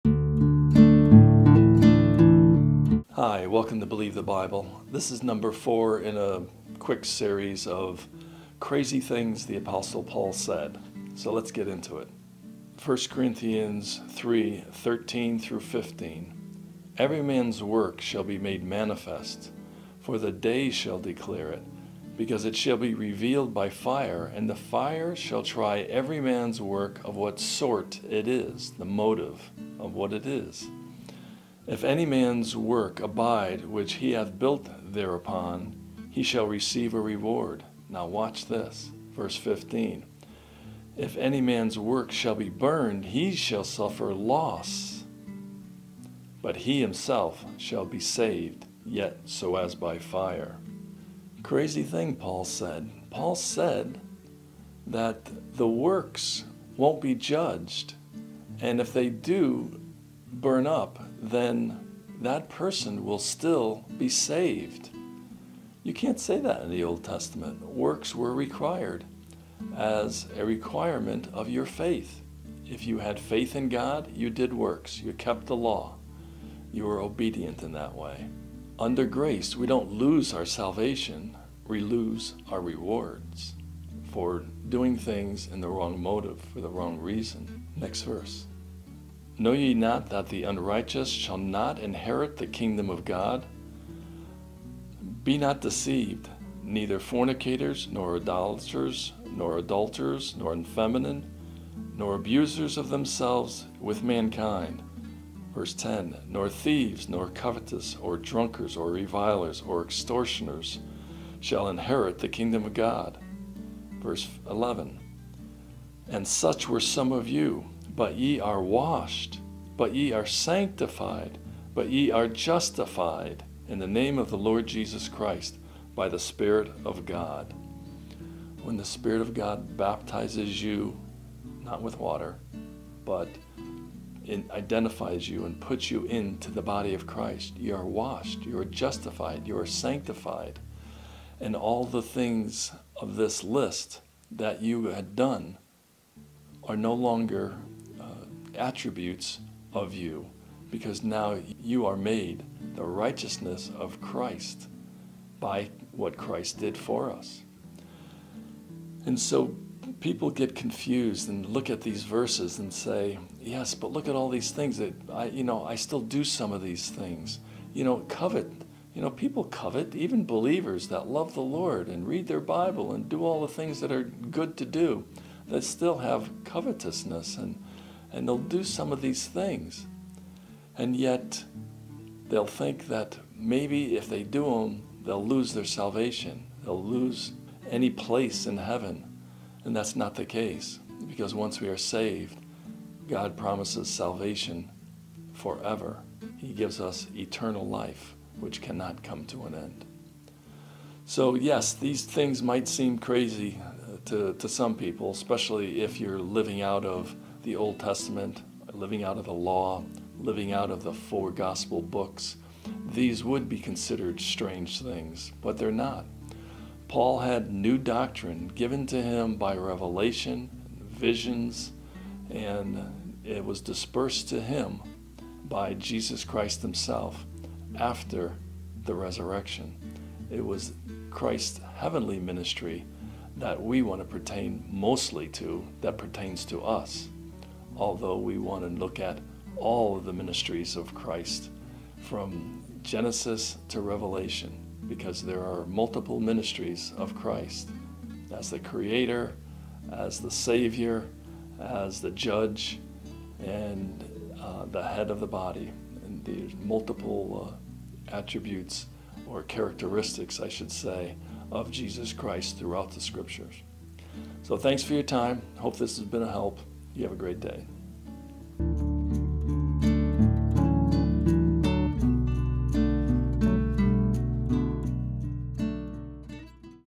Crazy things Paul said #5
We know nothing Paul said was crazy to the rightly divided bible student.  But to those who think there is only one gospel and live out of the 4 gospel books they will seem strange.  Sad but true.